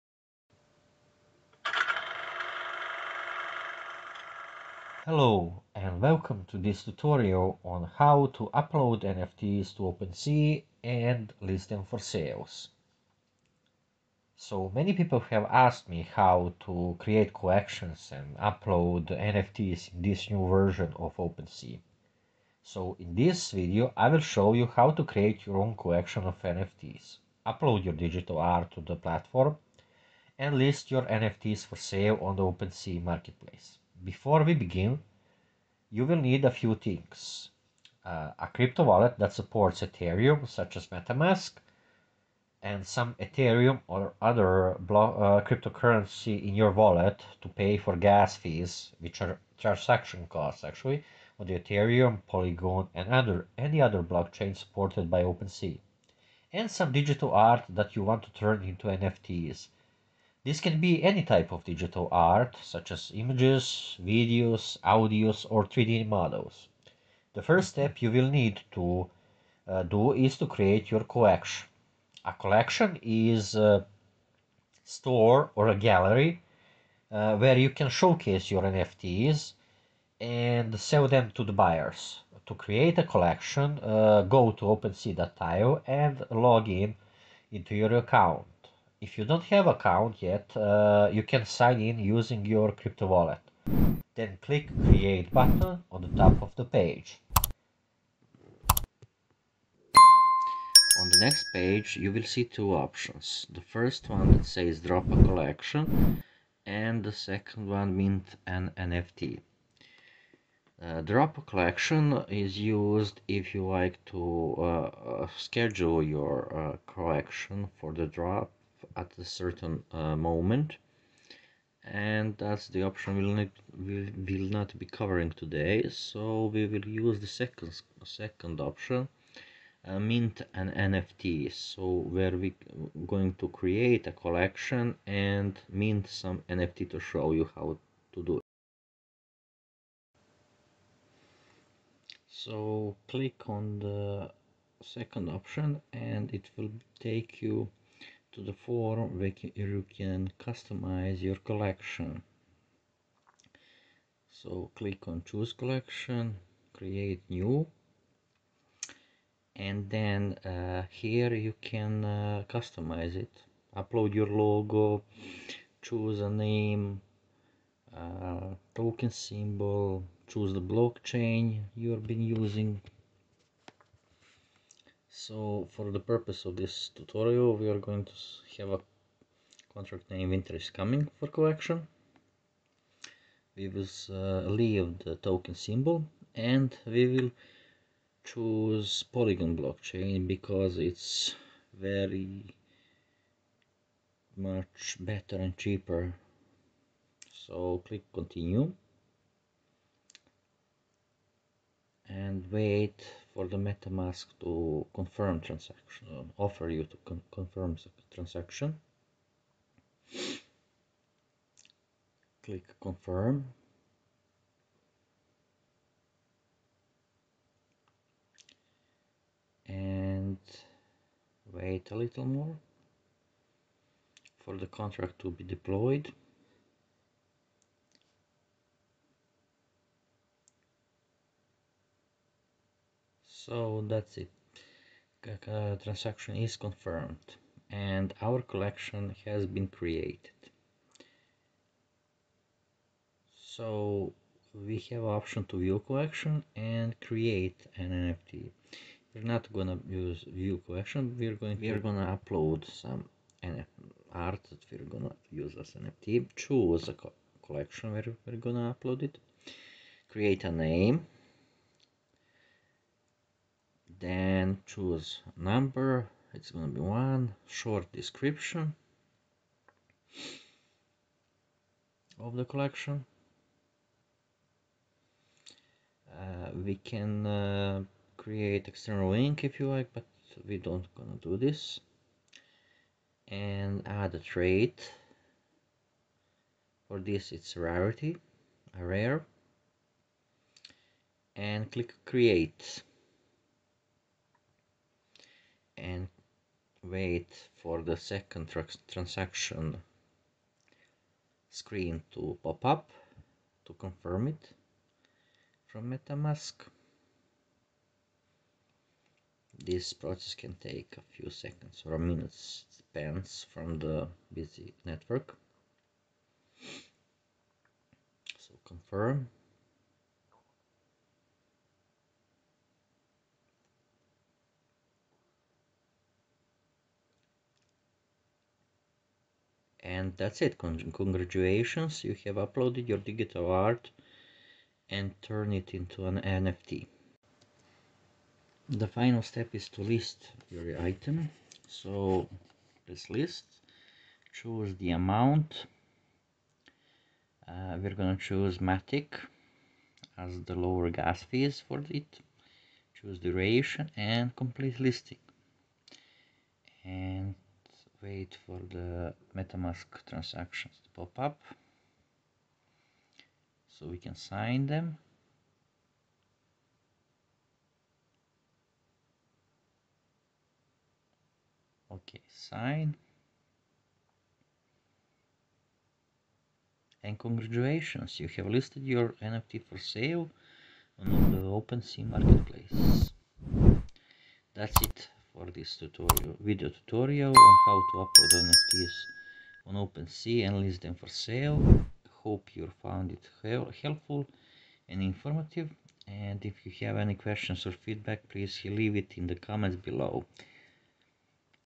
🎨How to Create NFT Collection and mint NFT on the new version of OpenSea 2023: A Step-by-Step Guide🌟
Learn how to create your own NFT collection and upload it on the new version of OpenSea (2023) with this comprehensive tutorial (collections shown via Studio). This step-by-step guide will walk you through the process of creating a collection, minting NFTs, and uploading them on the blockchain. You'll also learn how to list your NFTs on OpenSea.
Start selling your NFTs today!

Also, explore the latest in Web3 with posts on NFTs, cryptocurrency, blockchain, digital innovation and more